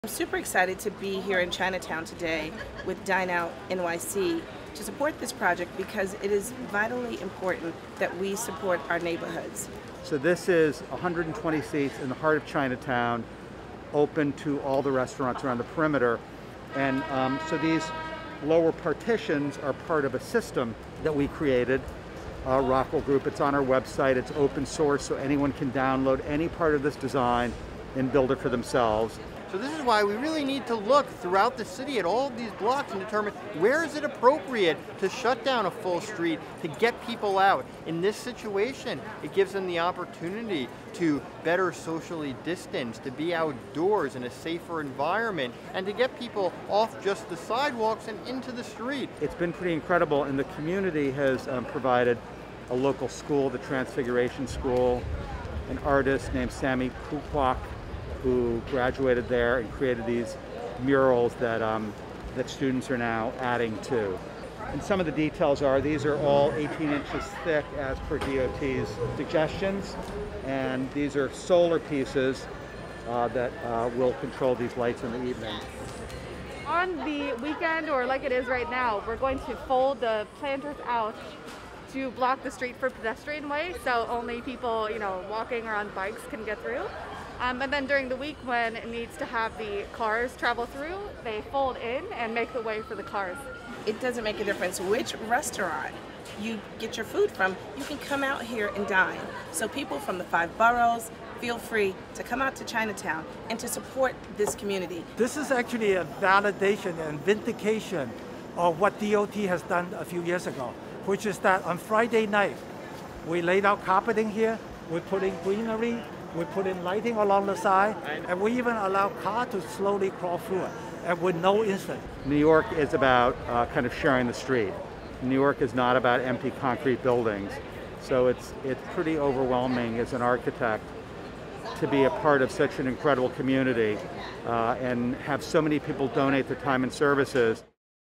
Chinatown’s Mott Street Restaurants Get Car-free Street Dining
Now you can see car-free Mott Street with your own eyes.

Streetfilms auteur Clarence Eckerson filmed the official launch on Wednesday of the new open restaurant street in Chinatown between Worth and Mosco streets — and found more than 100 safe, comfortable seats in multiple dining areas along curbs that up until now had been used only by vehicles.

Boosters say Mott Street points the way to what can be done citywide.

“We really need to look all over the city at all these blocks and determine where it is appropriate to shut down a full street,” said Andrew Rigie of the NYC Hospitality Alliance.

Longtime restaurant architect David Rockwell was also on hand to see his handiwork.

“New York is about sharing the street — it’s not about empty concrete buildings, so it’s pretty overwhelming as an architect to be a part of such a wonderful community,” Rockwell said.

Rockwell’s company, The Rockwell Group, created the modular designs and is providing them for free on the company’s website.

One nifty design feature are rolling shelves filled with plants that can be pulled out from the seating areas to fully close the roadway to cars every Friday night.

NYC Hospitality Alliance President Melba Wilson saw a bigger goal than just eating.

“It is vitally important that we support our neighborhoods,” said Wilson, owner of her eponymous restaurant in Harlem.

Also watch the two-minute film for a blink-and-you’ll-miss-it cameo by Streetsblog Editor/Recovering Shoulder Separation Victim Gersh Kuntzman and super intern Adam Light.